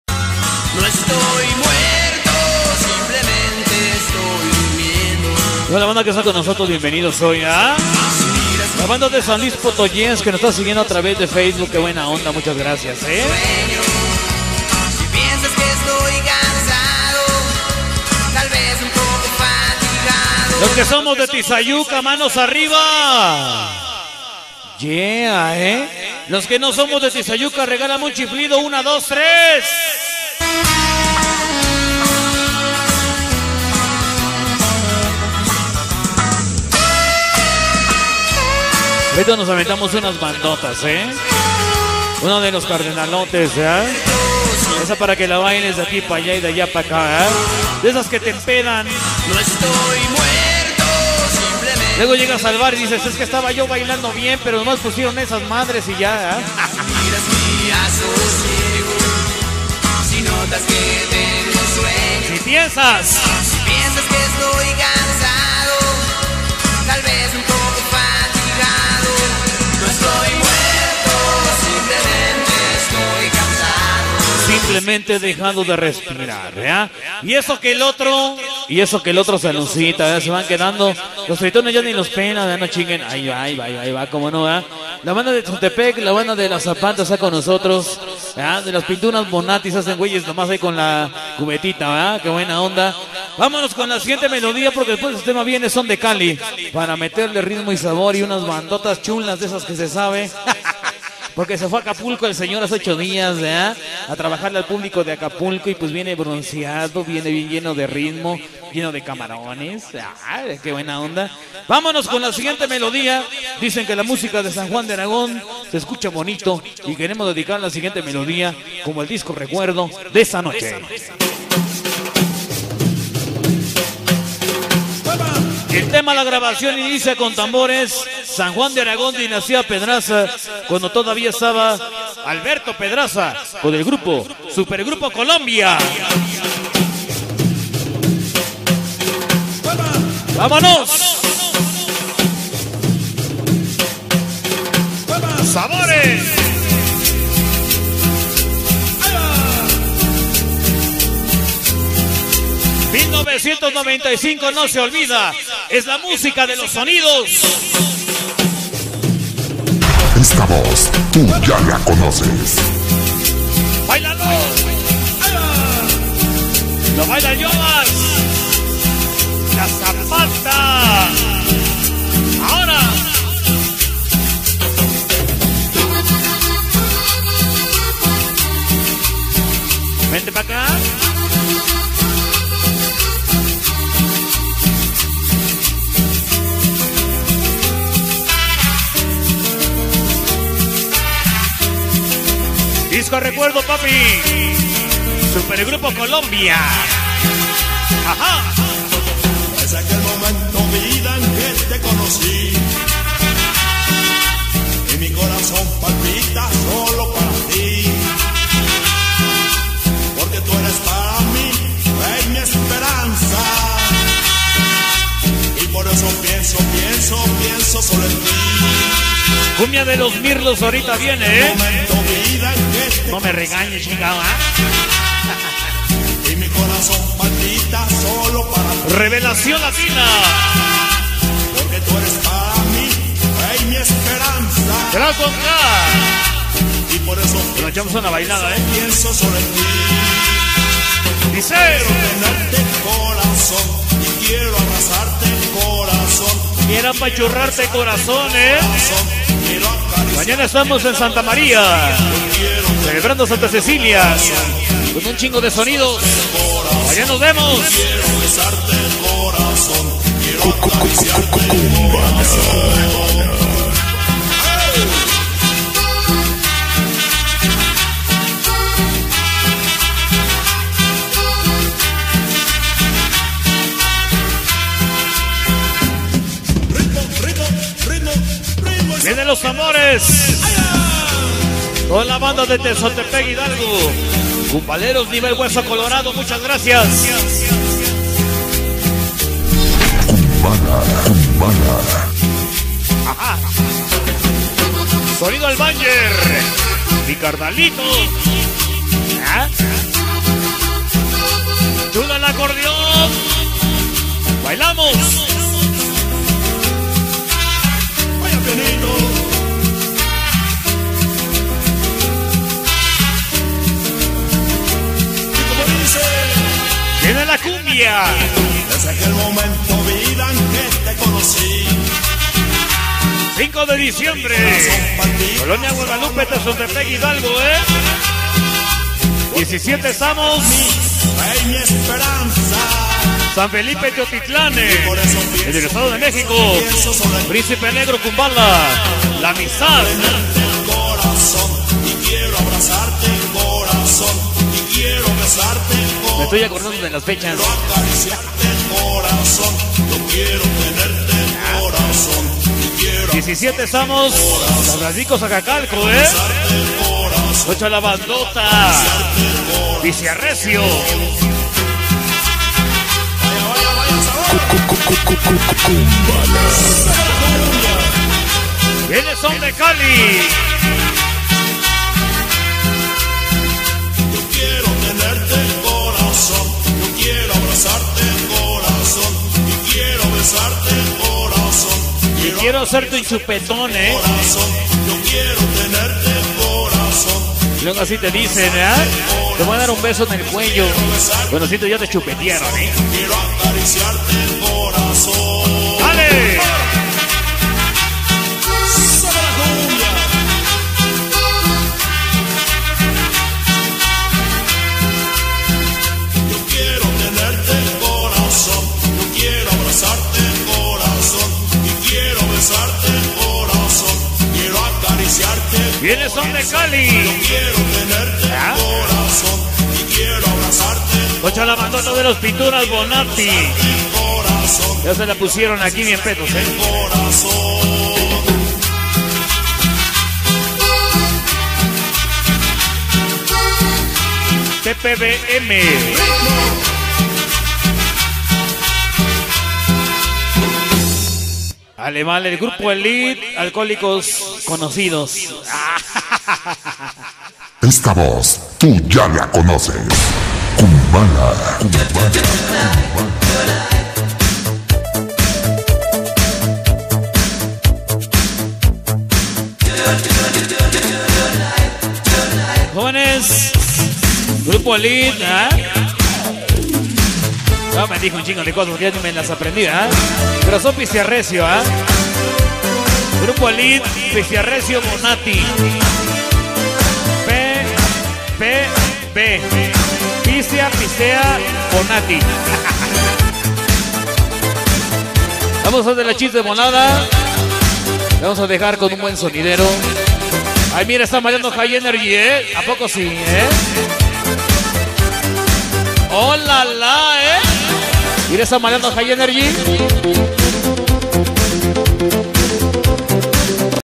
Sonido Kumbala
Algunos temas tocados, escuchados y bailados en bar mi tierra, tizayuca hgo.  con sonido kumbala